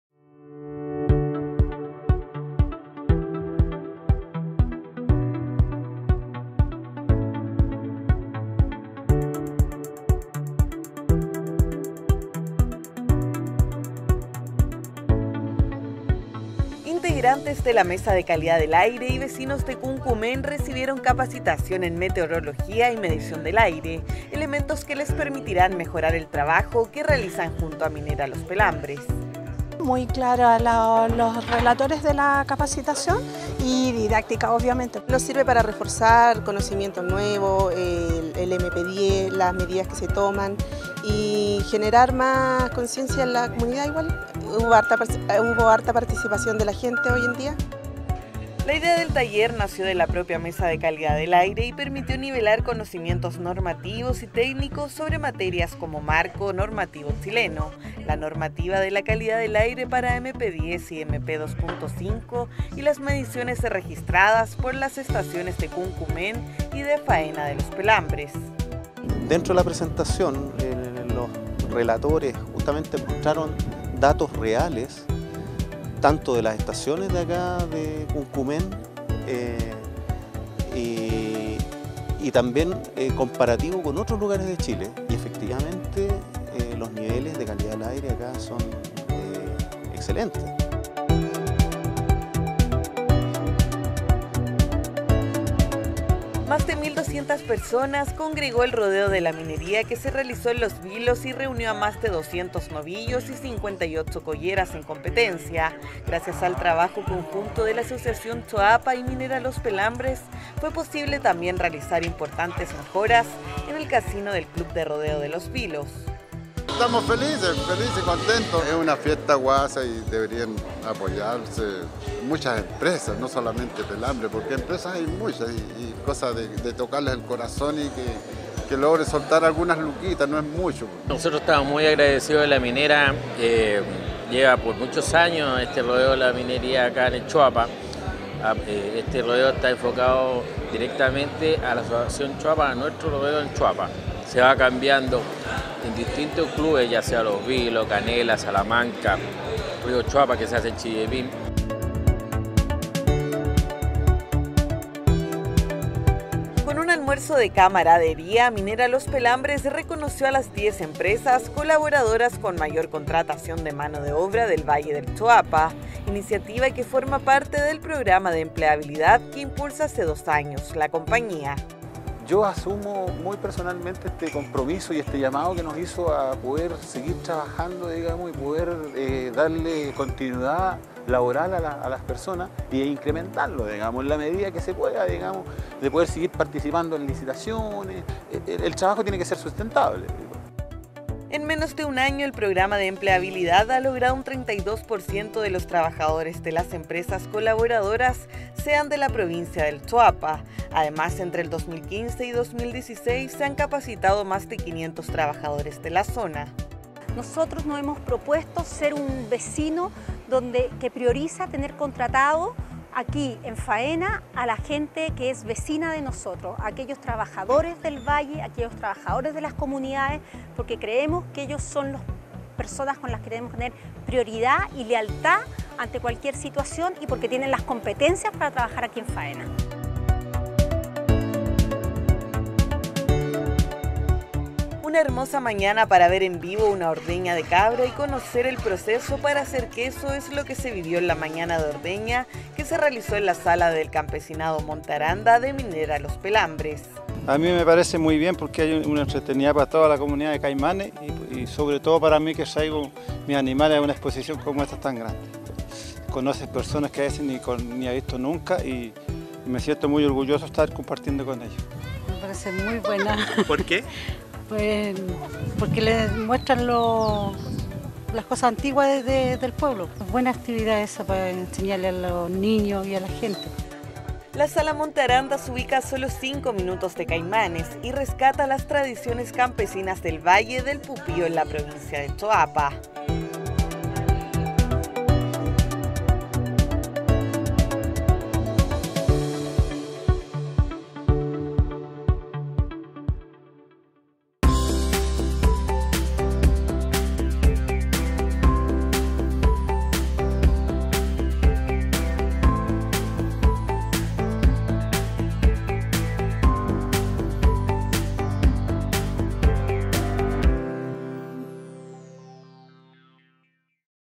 Notas Breves MLP - Octubre
- Taller de nivelación en conocimientos sobre calidad del aire.
- Exitosa nueva versión del Rodeo de la Minería.
- Reconocen a Empresas Colaboradoras que incorporan a trabajadores del Choapa.
- Mañana de ordeña en Monte Aranda.